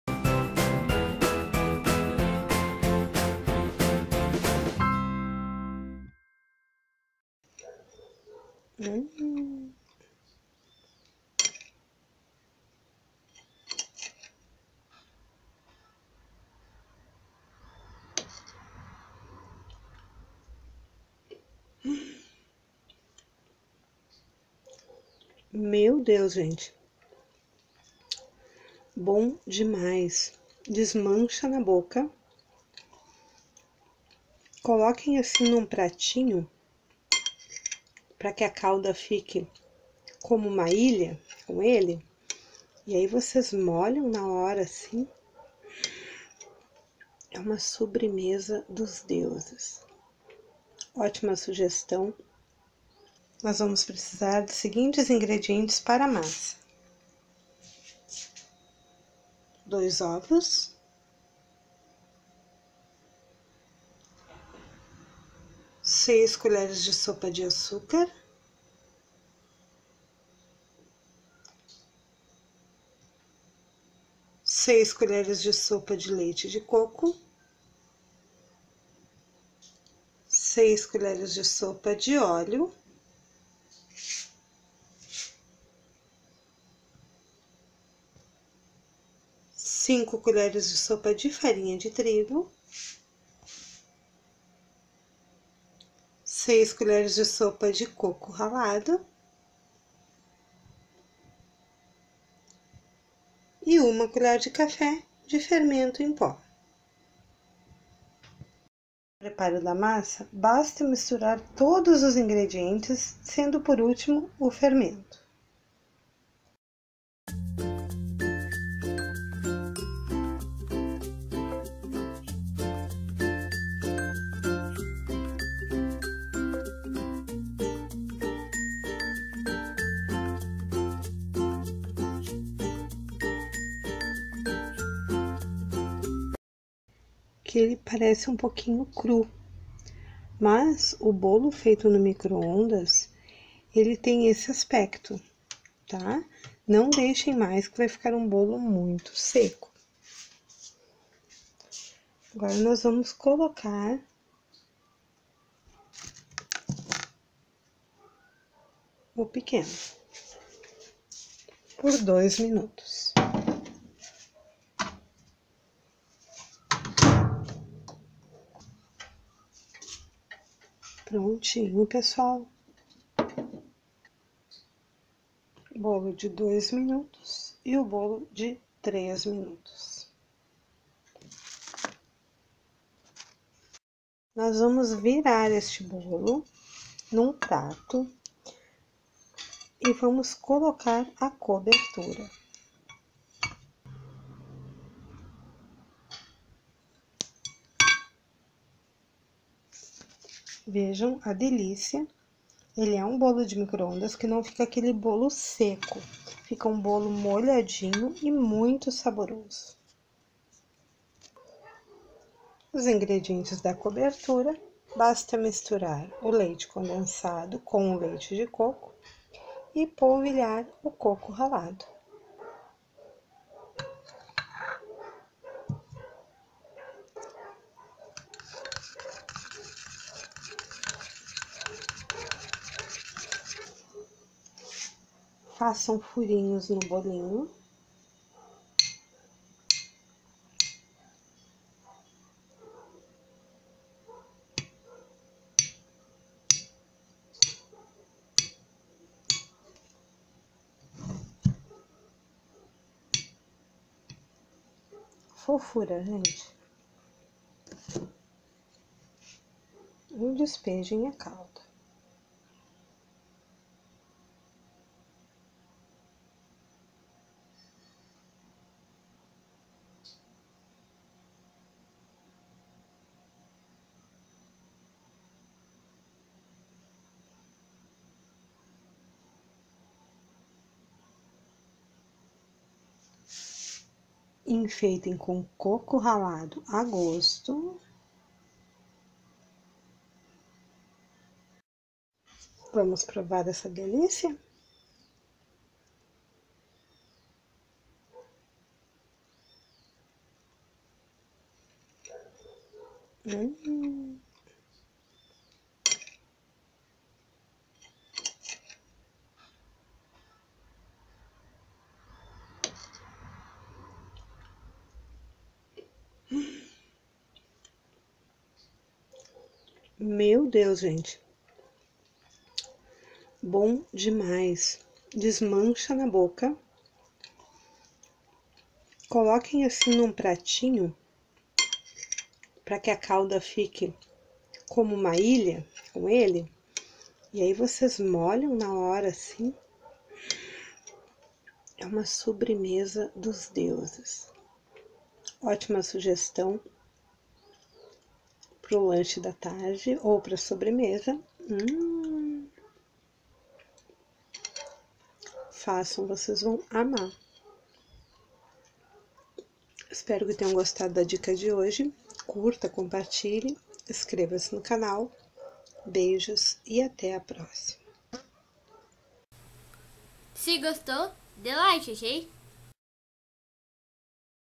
Toalha Felpuda de Caneca de 3 minutos!!
Bolos feitos no microondas tem fama de serem secos e sem gosto neh?Mas depois dessa receita seu conceito nunca mais será o mesmo!!
INGREDIENTES DA MASSA:
RENDE 2 CANECAS
6 COL. SOPA DE LEITE DE COCO
6 COL. SOPA DE ÓLEO
6 COL. SOPA DE COCO RALADO
6 COL. SOPA DE AÇÚCAR
5 COL. SOPA DE FARINHA DE TRIGO
1 COL. CAFÉ DE FERMENTO EM PÓ
2 OVOS
COBERTURA:
5 COL. SOPA DE LEITE DE COCO
4 COL. SOPA DE LEITE CONDENSADO